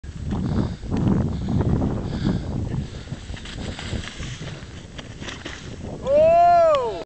Snowbike tailwhip nofoot cancan
Nicolas trying tailwhip no foot cancan. Cancan needs to be a bit more stylish and he landed it a bit too short.